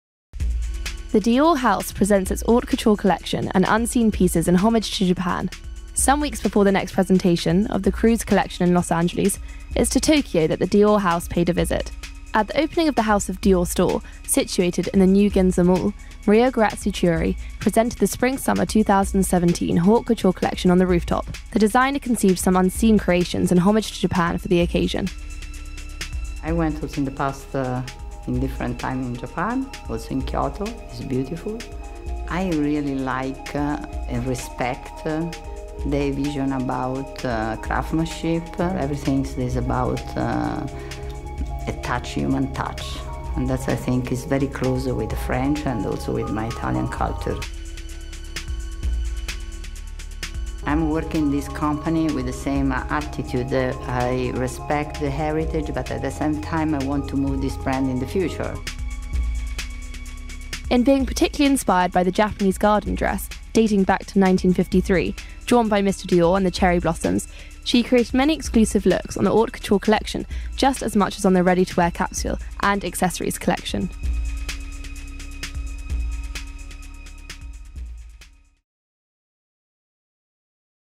東京で初披露ルック、マリア・グラツィア・キウリが語る
「ディオール（Dior）」はオートクチュールコレクションと、日本にオマージュを捧げた作品を発表した。同ブランドのアーティスティックディレクター、 マリア・グラツィア・キウリ（Maria Grazia Chiuri）に話を聞いた。(c)Parismodes.tv